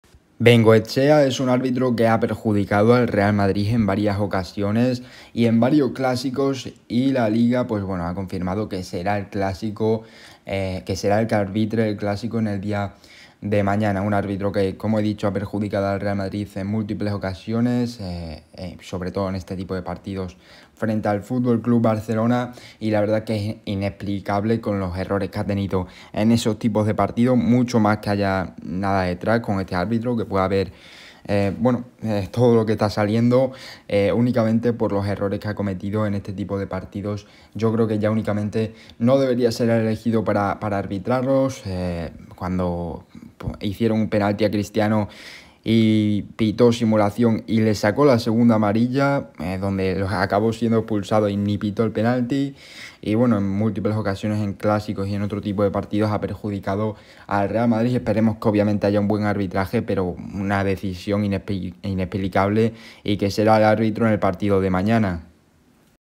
¡LA LIGA LO CONFIRMA! ¡EL MADRID EXPLOTA! ¡GRAN ENFADO! | NOTICIAS DEL REAL MADRID HOY
Ricardo De Burgos Bengoetxea volverá a dirigir otro clásico en su carrera. Lo hará este domingo, tal y como ha anunciado la RFEF en sus designaciones para la jornada 26ª de LaLiga. Noticia que ha sentado como un jarro de agua fría en el Real Madrid.

No es ningún secreto que el colegiado vasco tiene un historial bastante negativo contra el Real Madrid. Inolvidable su clásico de la Supercopa de España en la que expulsó a Cristiano y señaló un penalti de Navas a Suárez inexistente.

En el Real Madrid no han tardado en reaccionar al conocer que Bengoetxea será el árbitro del clásico. Se preguntan cómo es posible que árbitros con gran bagaje internacional como Gil Manzano o Mateu Lahoz casi nunca reciban estos partidos.

José Luis Sánchez, de 'El Chiringuito', desvela las estadísticas y son bastante llamativas. Mateu Lahoz, que incluso estuvo en el Mundial de Qatar, sólo ha dirigido 5 clásicos desde el año 2008.

Curioso
Mientras que Gil Manzano, otro árbitro muy bien valorado en la FIFA, sólo 2 desde el año 2012. Una alta responsabilidad, la de estos partidos, que recae en colegiados con informes alejados del sobresaliente y con poca experiencia.

Es habitual ver en los clásicos a árbitros como Hernández Hernández, De Burgos Bengoetxea, y Sánchez Martínez. Este fin de semana el foco estará en Bengoetxea y Soto Grado desde el VAR.